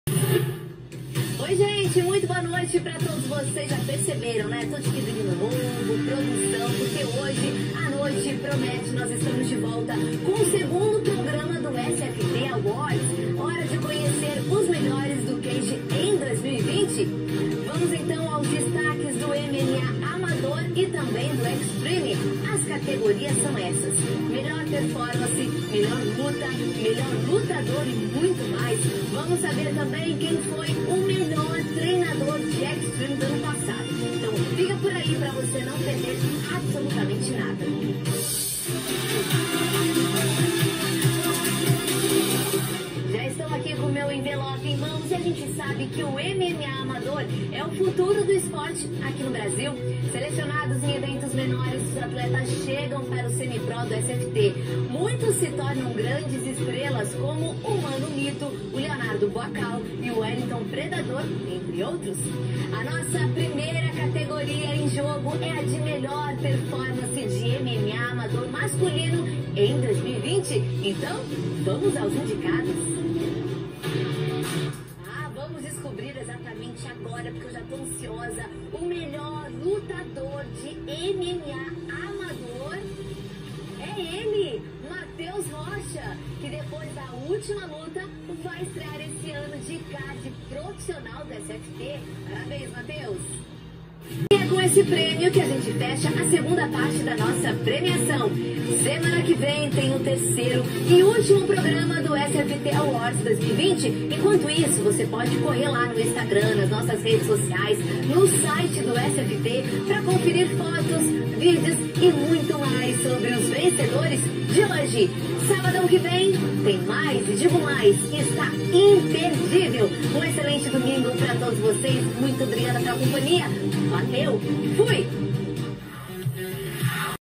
Mirelle Moschella SFT AWARDS 2021!
Mirelle Moschella apresenta o especial SFT AWARDS 2021. Os melhores atletas do ano de 2020 são premiados por várias categorias !